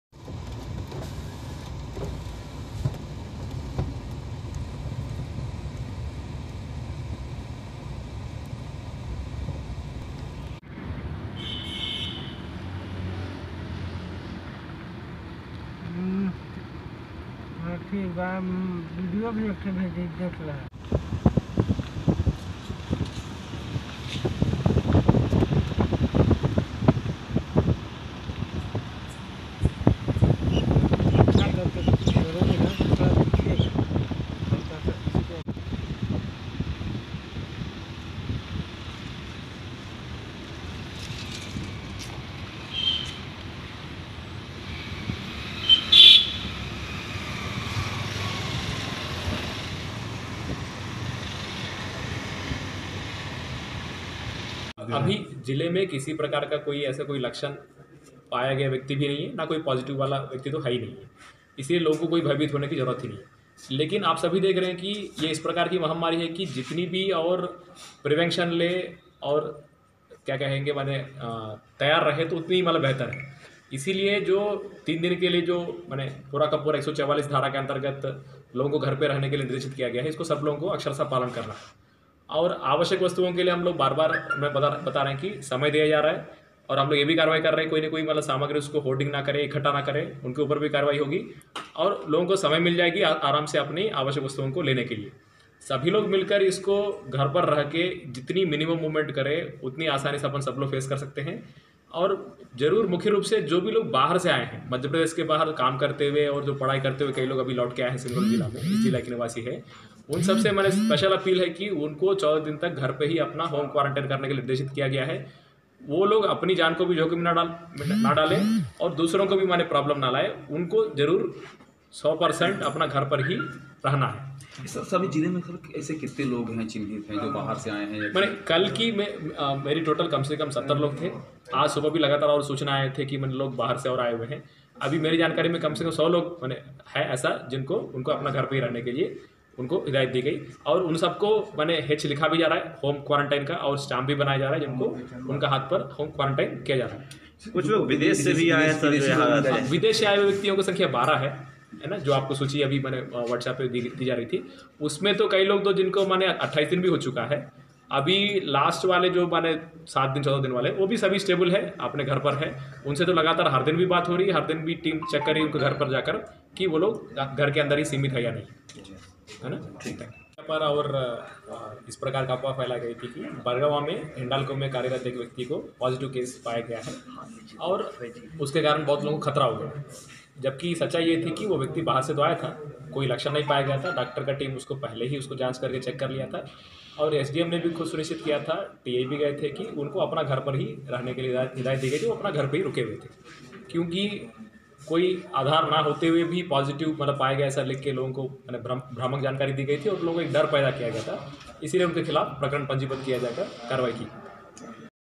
कोरोना संक्रमण की अफवाह फैलाना युवक को पड़ा महंगा, केस दर्ज
Vindhyanews